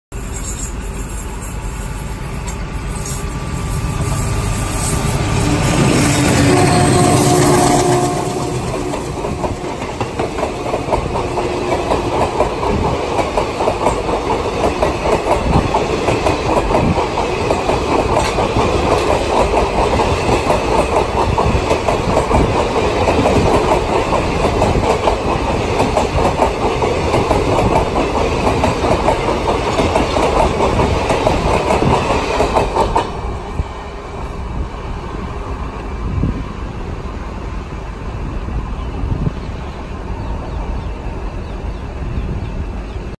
Close Encounter of Express Overtaking Shatabdi || Shatabdi Overtaken || Railways || MSA Repaints
First time in history an express train overtakes Shatabdi. Encountered early morning in travel diaries. By one and only MSA Repaints.